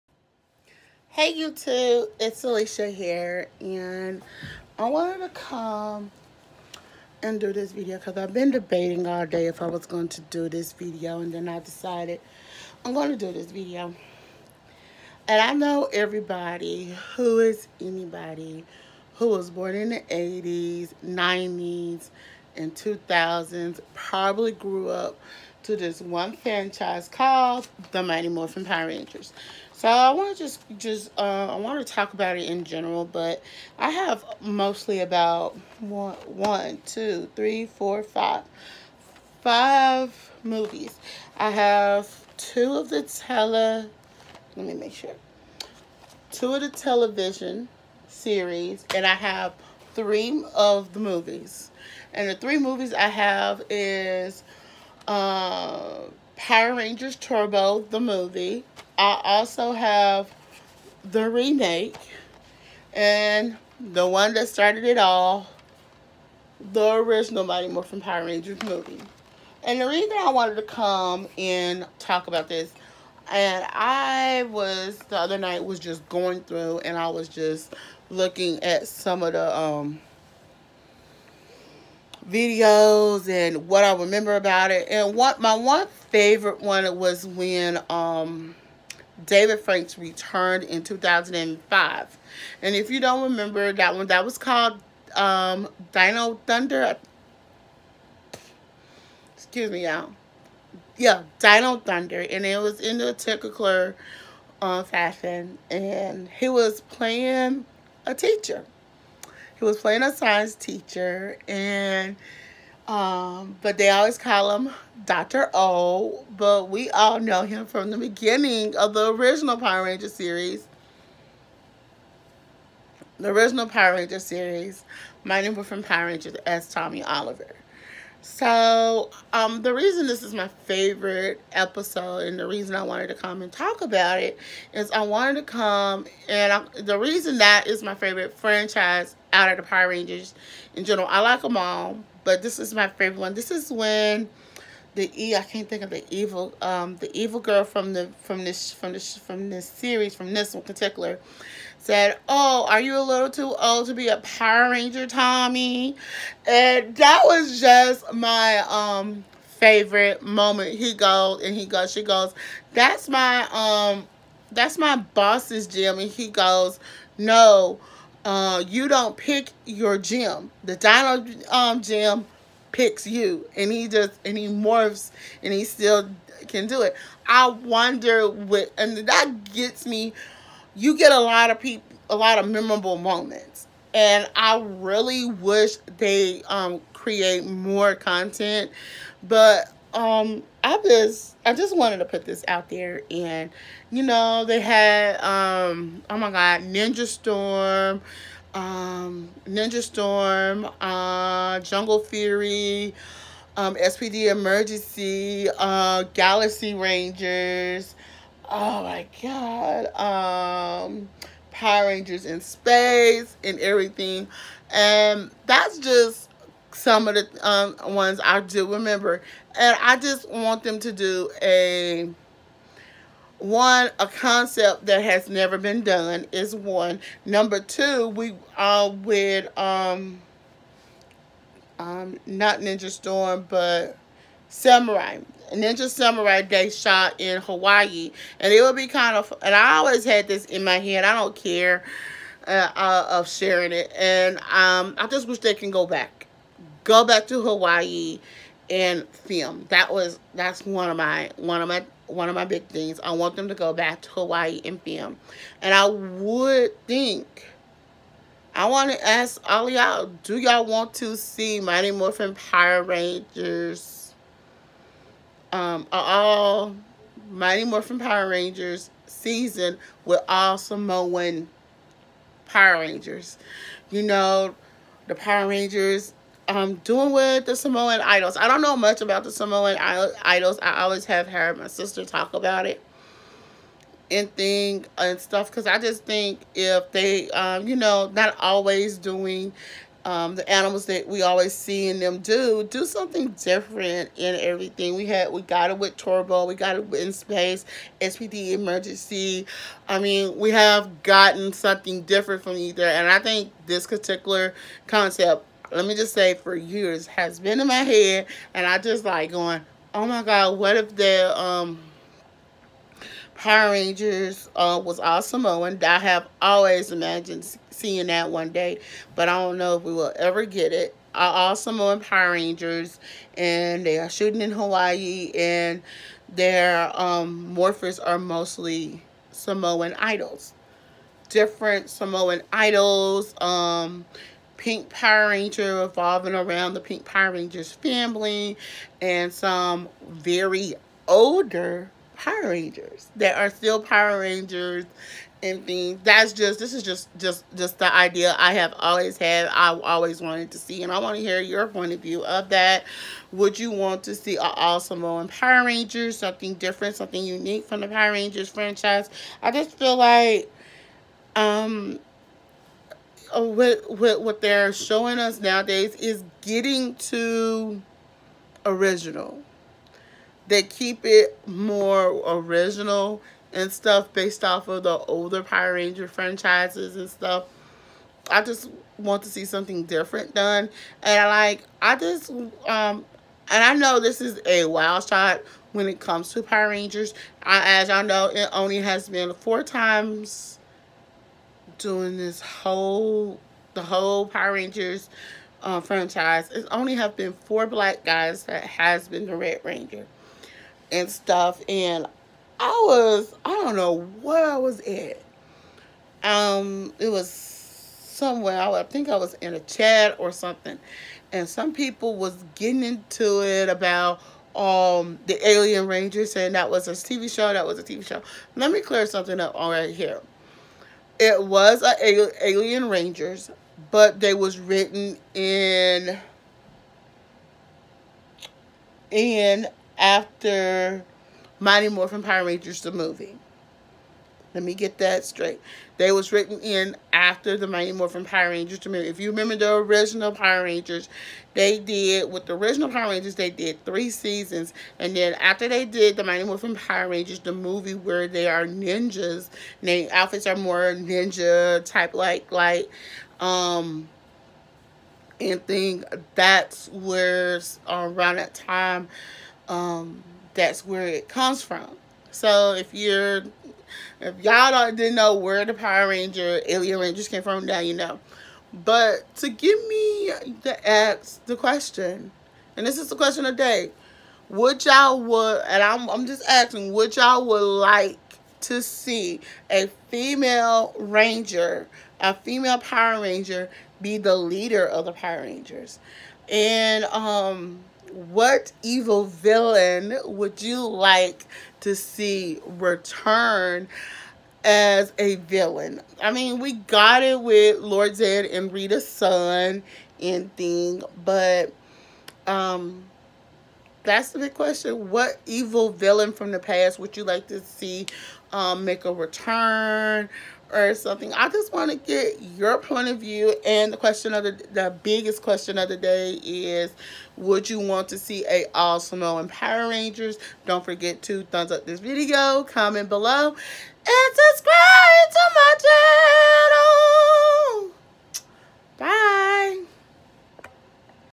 Mighty Morphin Power Rangers franchise
My Morphin Power Rangers franchise , this video is me describing what I would like to see in the next Power Rangers television series franchise just to see what they can do different with the franchise